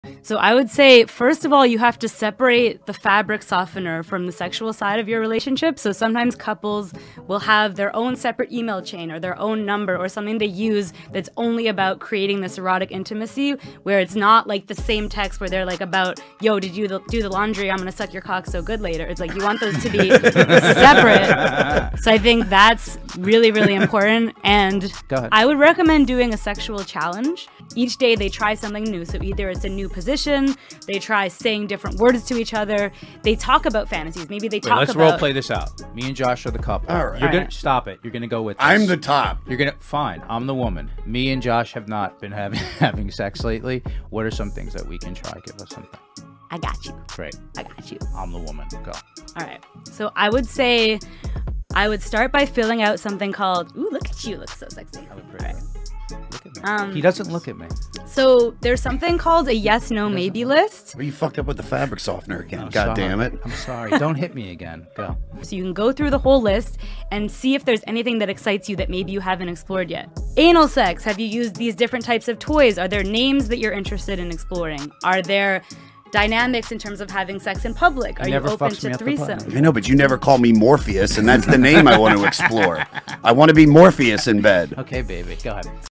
When Couples Stop 👉👌ing
Sex educator shares her tips for anyone with sexual anatomy on EP. 11